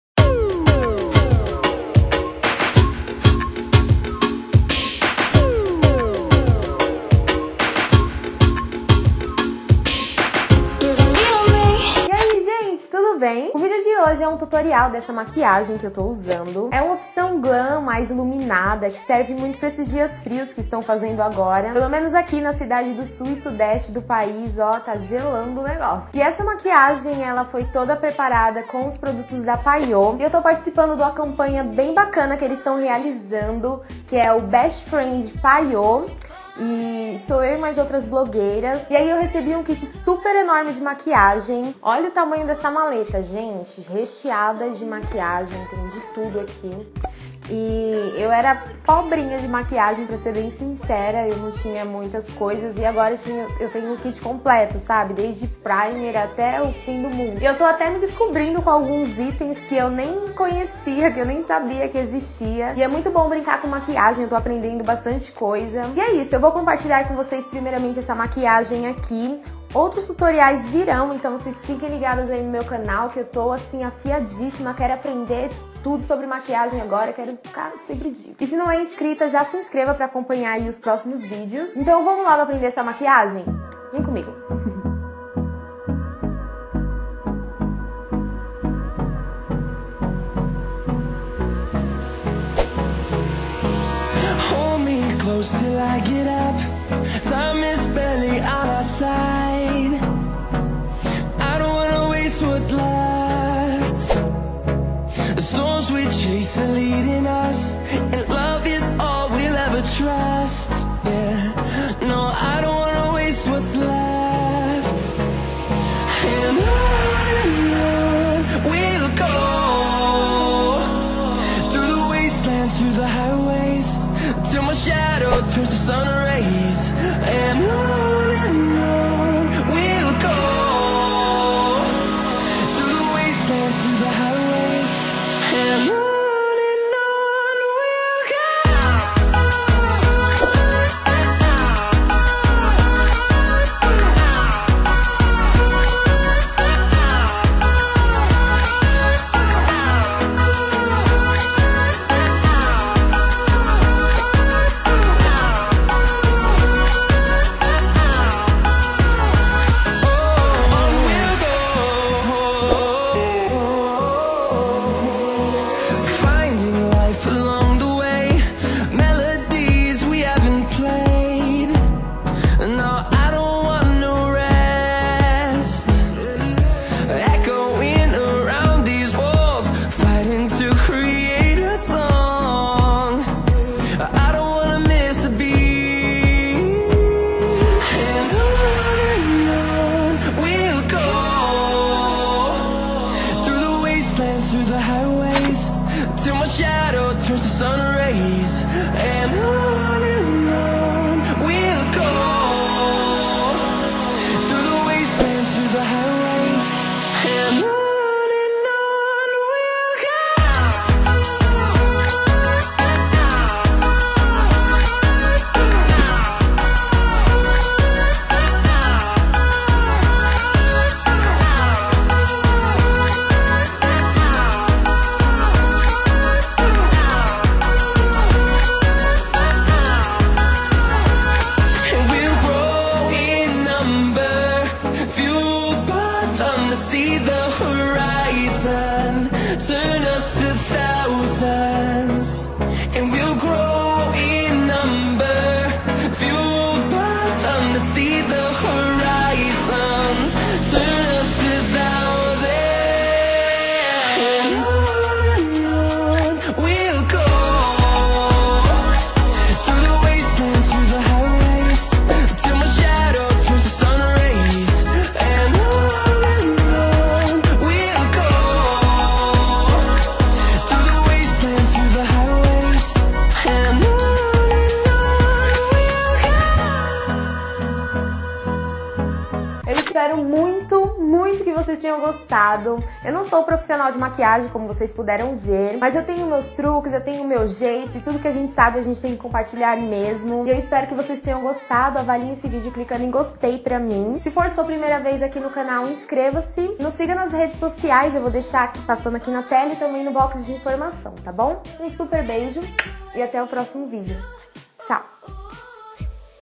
SOMBRA TERROSA, BATOM ESCURO + DELINEADO ESFUMADO
Quem gosta de maquiagem vai amar essa opção para usar em dias frios, pois a combinação de batom escuro + sombra terrosa dão um efeito glamour que cai super bem com um cachecol e roupas de frio de preferência na cor preta ( AMO )

Cx postal: 49330 Cep: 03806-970
São Paulo/SP

PERGUNTAS FREQUENTES:
Roteiro e produção por @wedie_